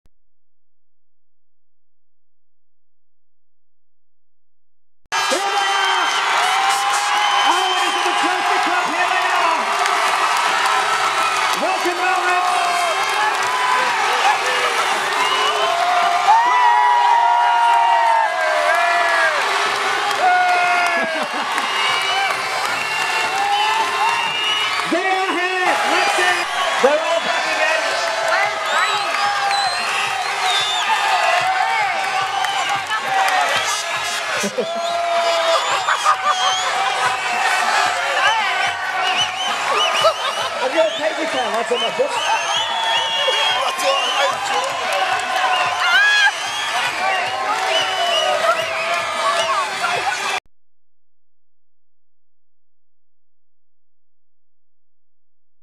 Barrow AFC Homecoming
Barrow AFC return to town with the FA trophy may 2010